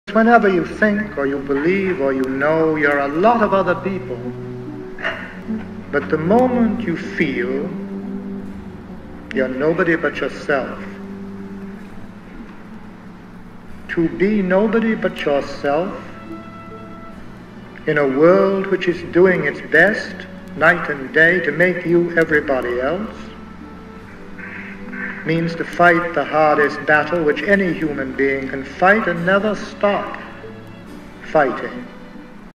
e e cummings: A Poet's Advice to Students
e e cummings, was an American poet, painter, essayist, author, and playwright. He wrote approximately 2,900 poems, two autobiographical novels, four plays, and several essays. He is often regarded as one of the most important American poets of the 20th century. Cummings is associated with modernist free-form poetry. Much of his work has idiosyncratic syntax and uses lower-case spellings for poetic expression.